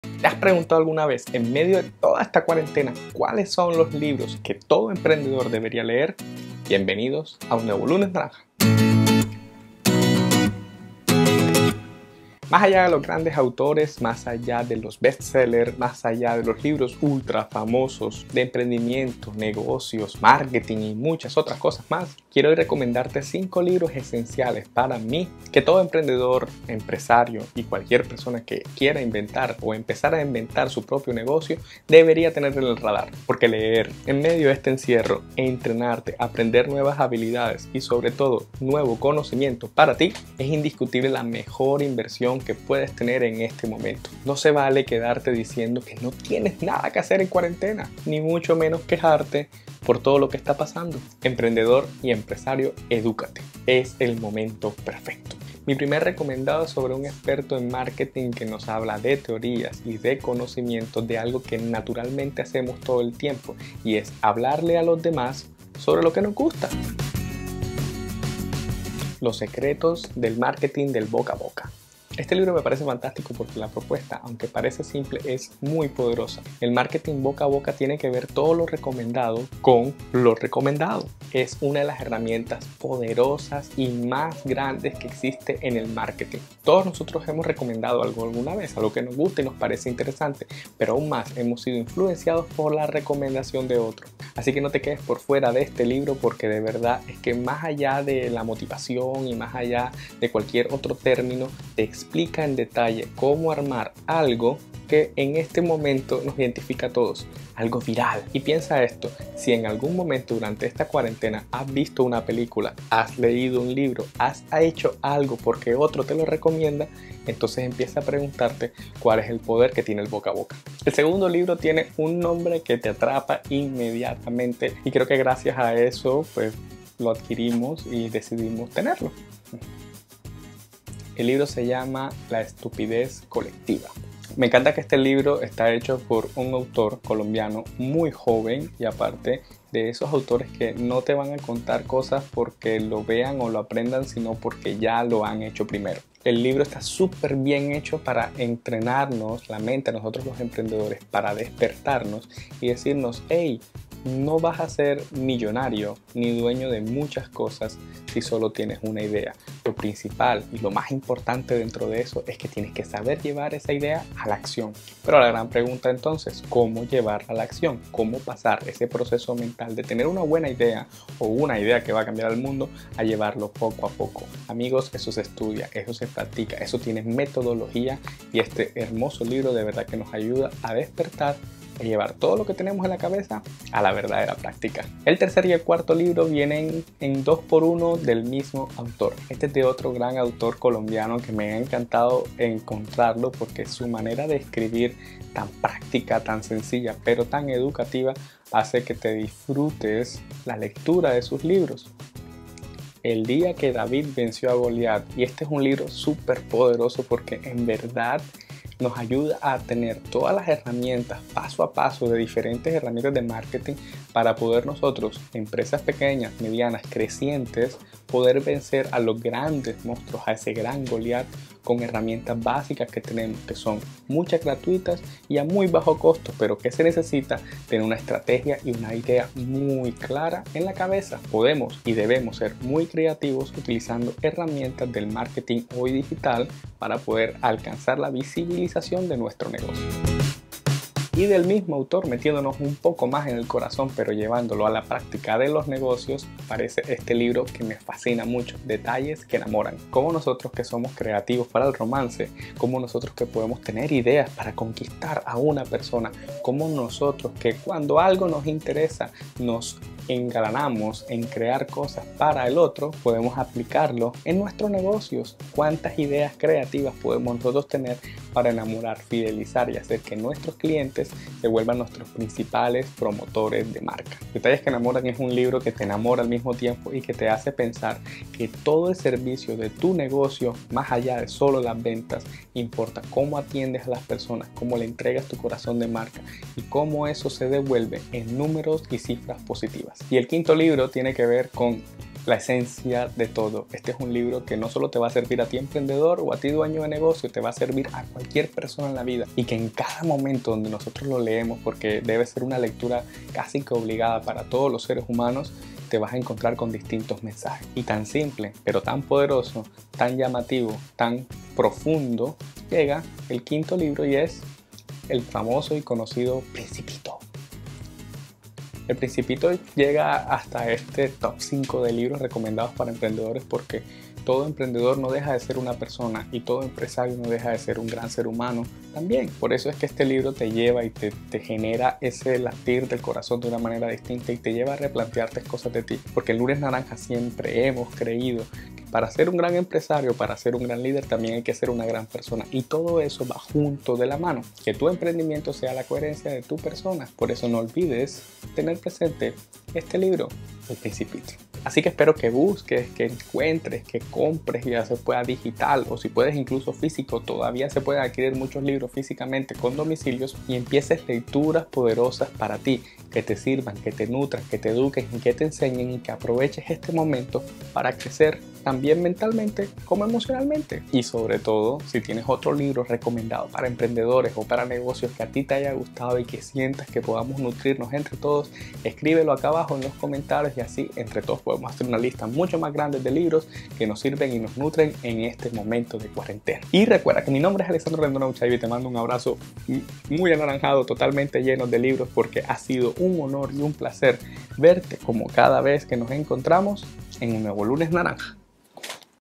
Mis 5 Libros para emprendedores en Cuarentena
Te quiero compartir mis 5 libros para emprendedores, perfectos para leer en esta cuarentena!!

Los secretos del marketing boca a boca - Geroge Silverman
La estupidez Colectiva - Julian Torres Gomez
El día que David venció a Goliat - David Gomez
Detalles que enamoran - David Gomez
El principito - Antoine de Saint-Exupéry

Cuál de estos conoces??
Recomiéndanos el libro ideal para ti en esta cuarentena!!